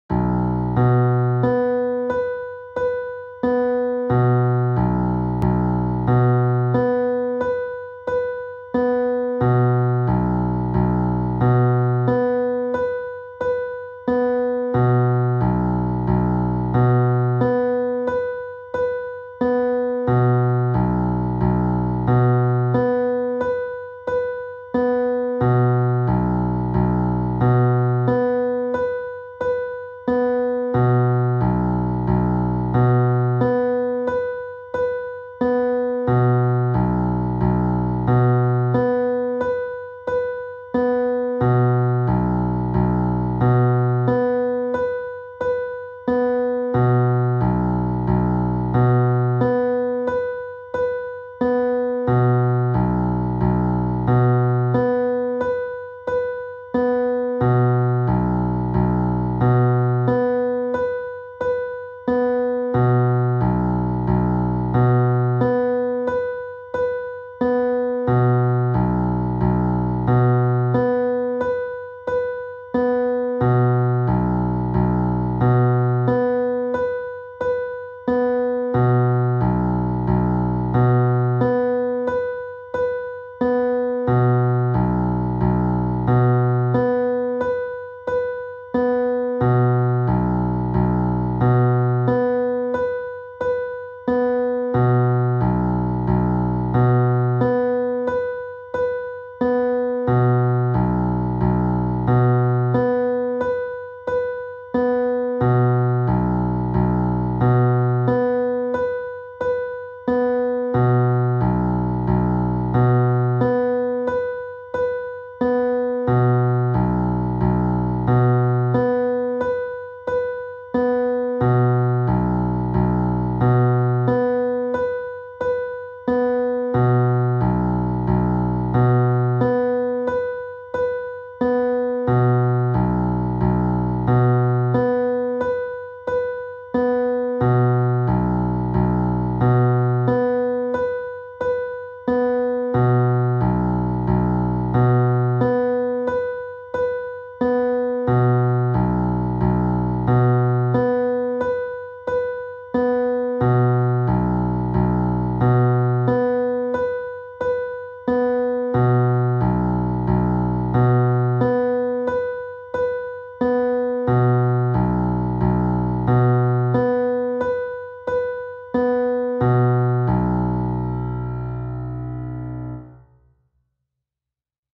B | Alternating Octaves | Four Octaves of B
Four octaves of B alternating on piano.